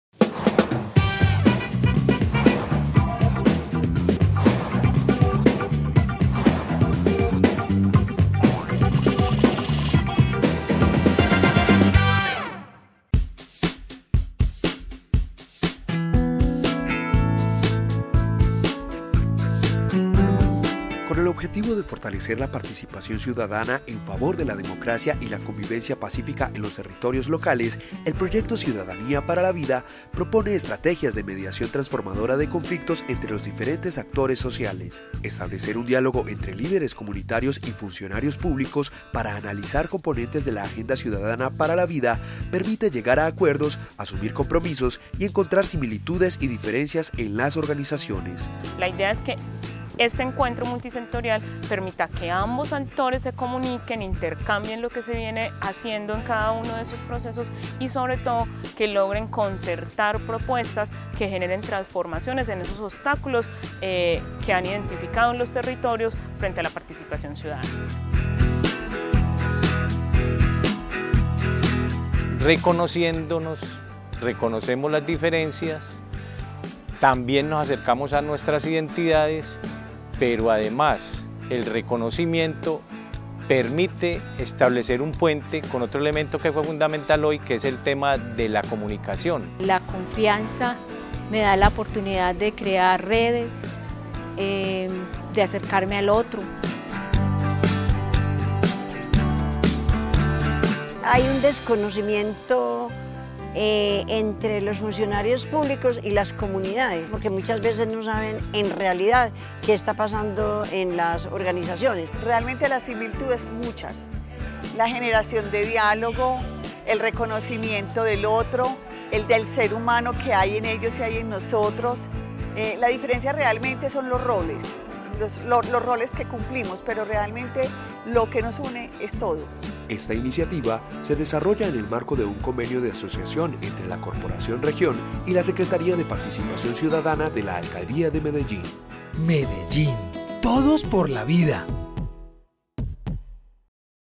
Diálogo multisectorial 15 de octubre - Ciudadanía para la vida
Un espacio donde funcionarios públicos, organizaciones privadas y organizaciones comunitarias se encuentran para hablar de la construcción y las herramientas que se han creado de forma colectiva en cada proceso de formación de Ciudadanía para la vida, para resolver los conflictos y dificultades que se  presentan a la hora de vivenciar la participación ciudadana.